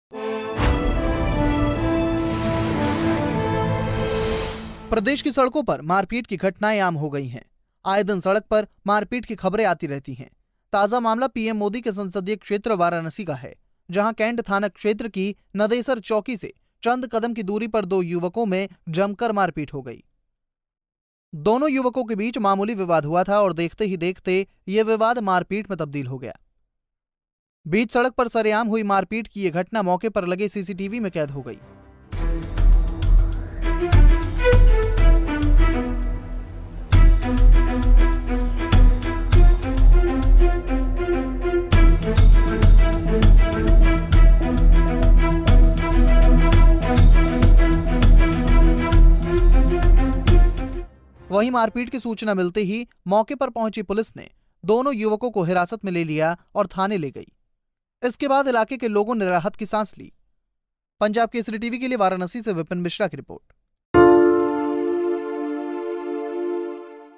मामूली विवाद में बीच सड़क पर भिड़े दो युवक, जमकर चले लात घूंसे
रदेश की सड़कों पर मारपीट की घटनाए आम हो गई हैं...आए दिन सड़क पर मारपीट की खबरें आती रहती हैं...ताजा मामला पीएम मोदी के संसदीय क्षेत्र वाराणसी का है..To Subscribe on Youtube